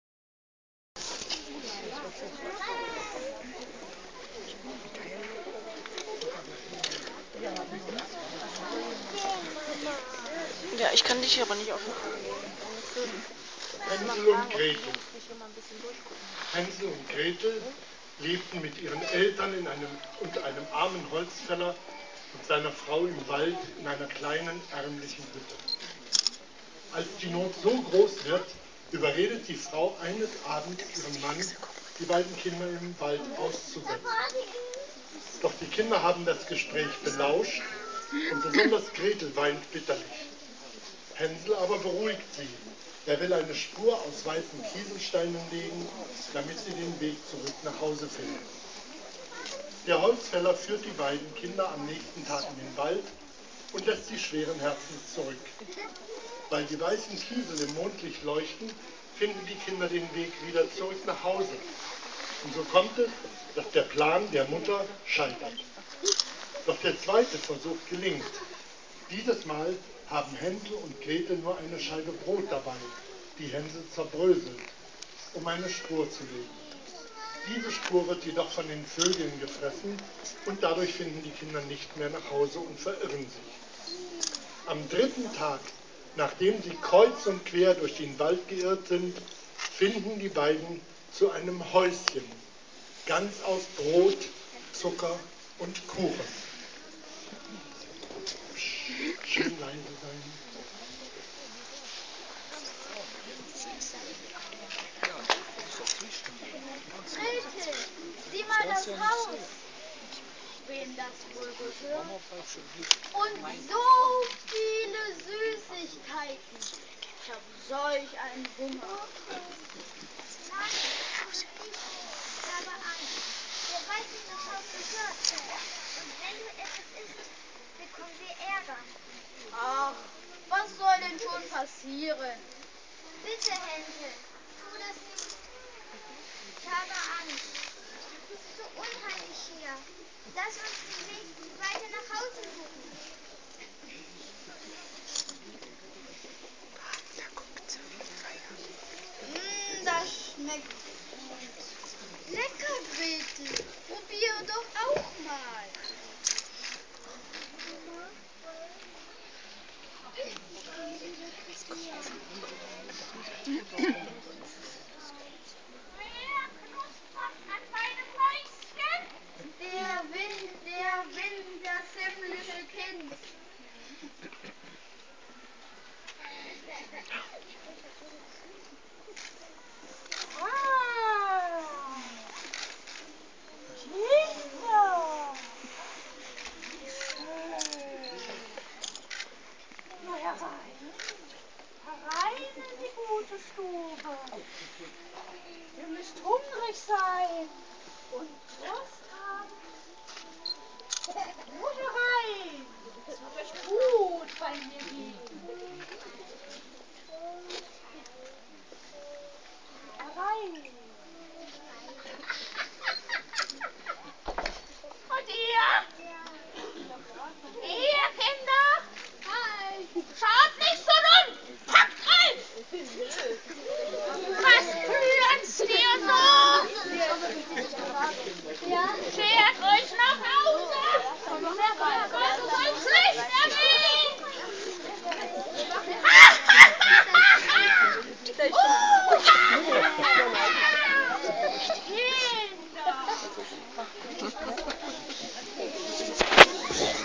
Adventskalender Teil 1
Erster Teil von Hänsel und Gretel
Teil 2 und 3 werden folgen.